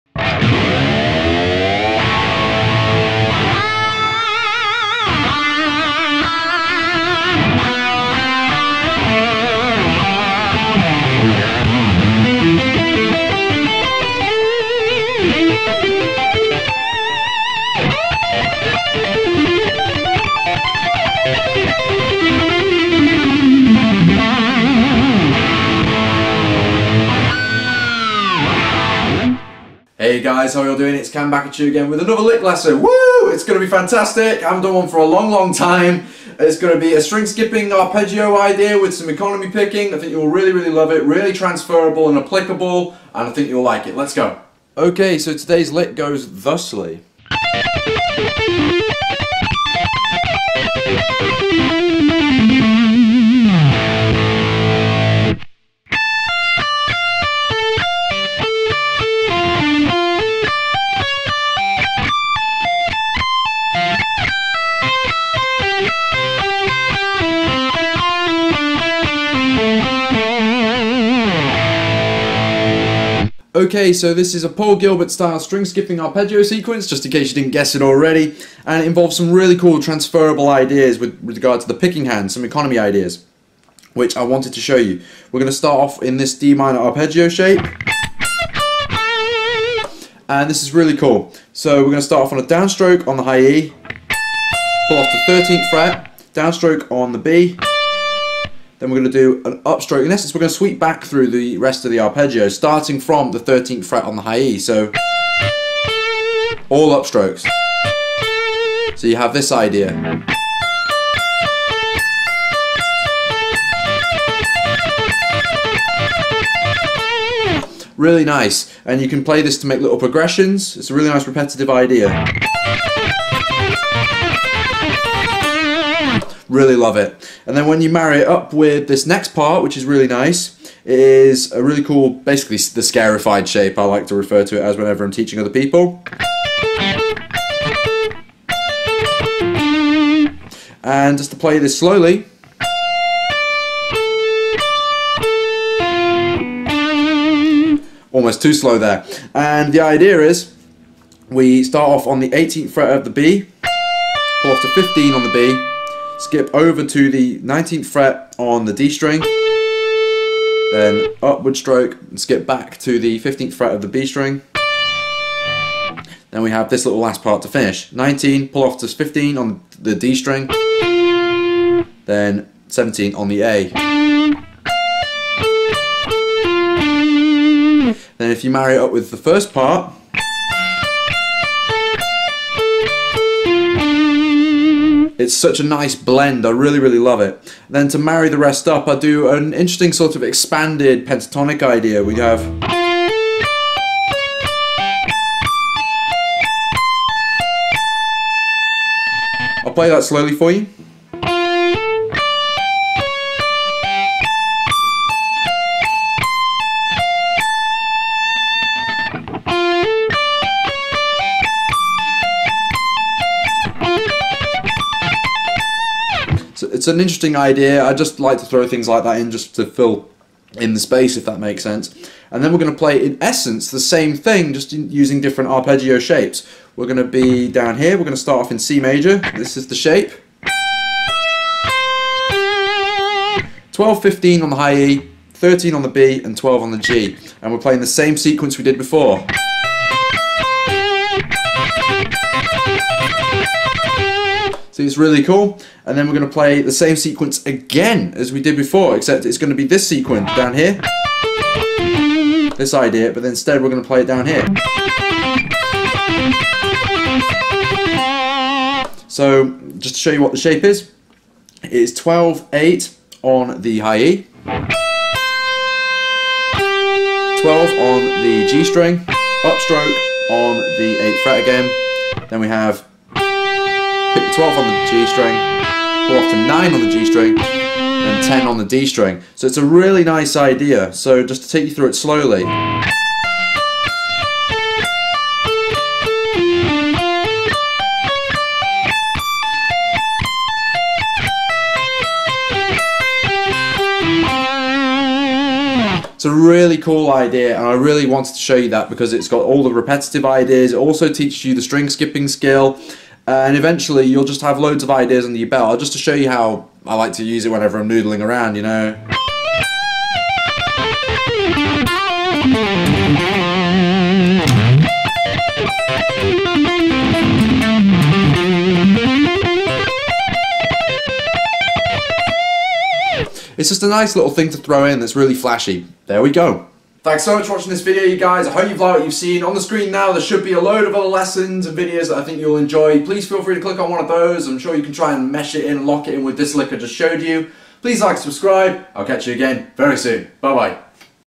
A CRAZY Way To Play Paul Gilbert Arpeggios
I thought I'd throw a challenge at you guys? Here are some really complex but useful arpeggio passages for you to get your teeth into.